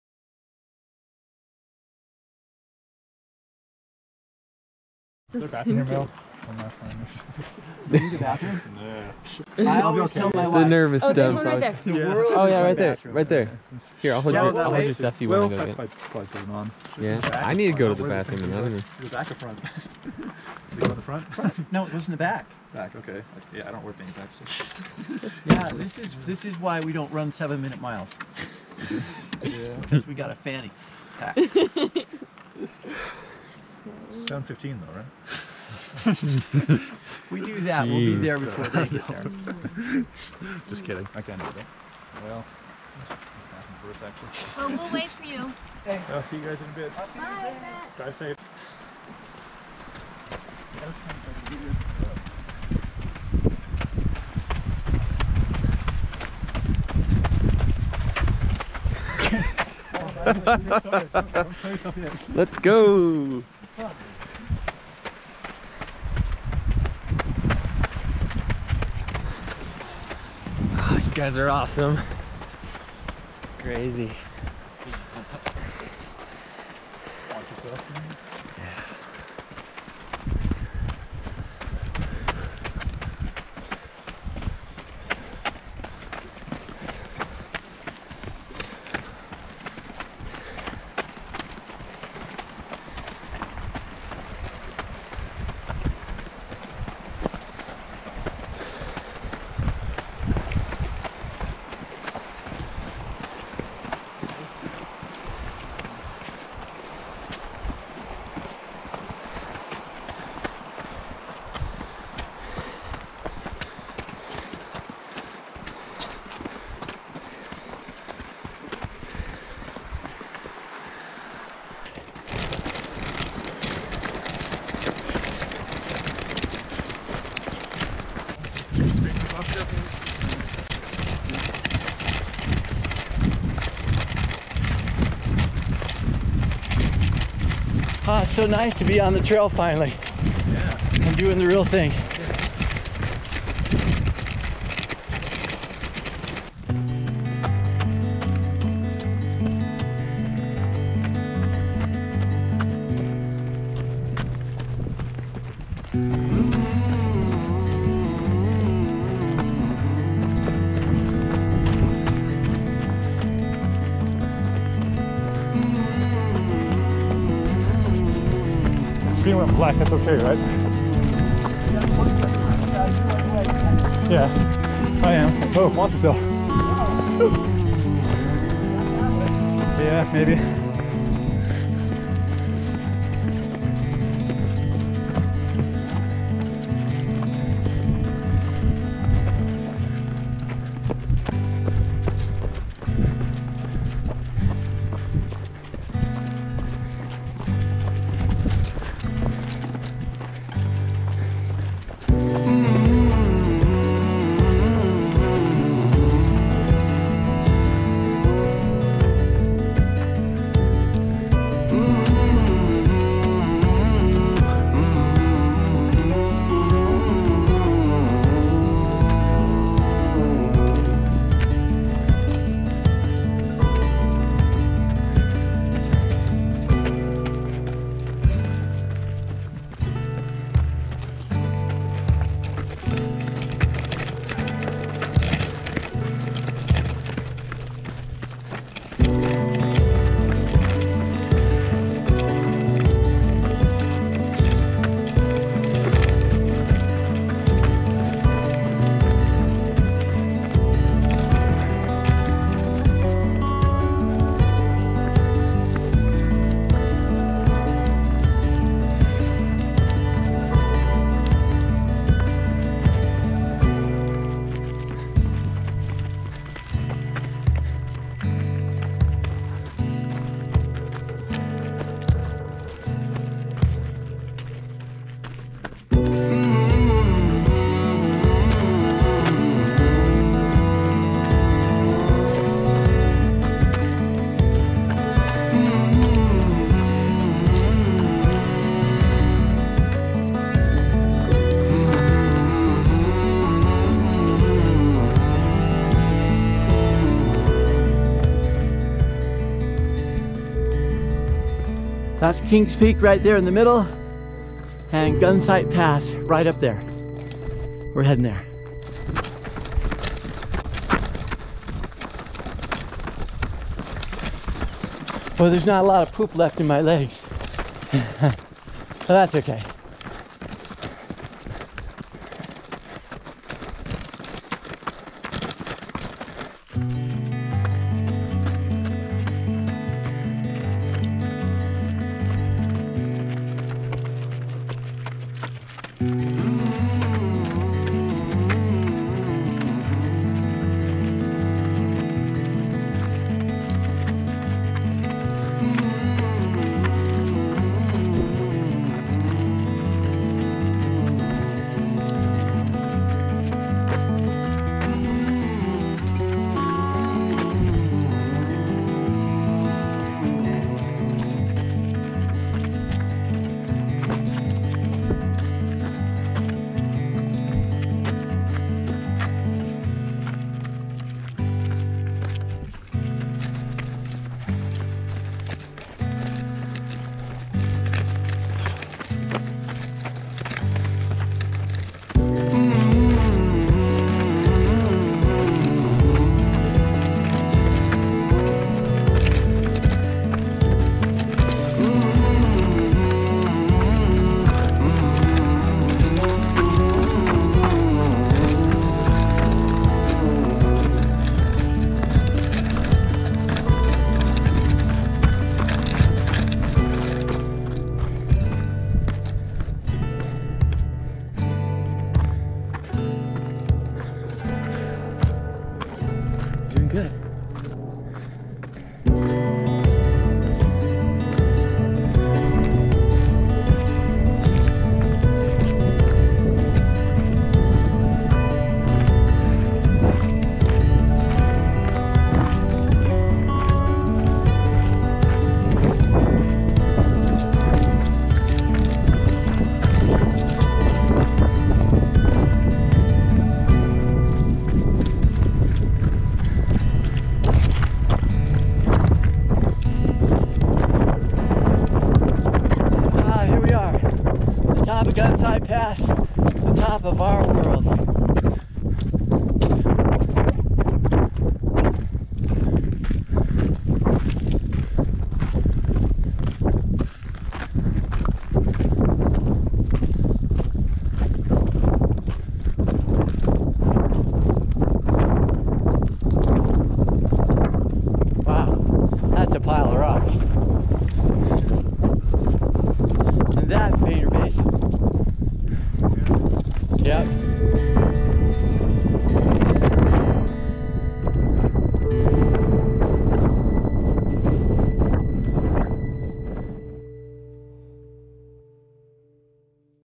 Race Day (Part 4) - Henry's Fork to Gunsight Pass
Solo Ultra Running across the mountains is the ultimate rush. With 40 miles of wilderness area in front of us, we start an unassisted run to cross the Uinta Mountains in one day or risk spending a freezing night wearing only running shorts.    Carrying a few emergency essentials we hope not to use, we plan to meet our support vehicle in 8-10 hours at the U-bar Ranch.  This is the first leg of our run to Gunsight Pass and the most difficult.  This section is well traveled by hikers going to King's Peak so if we need help, we can ask.   But there is no cell service or travelers in Painter Basin and I feel strangely naked not being able communicate to the outside world.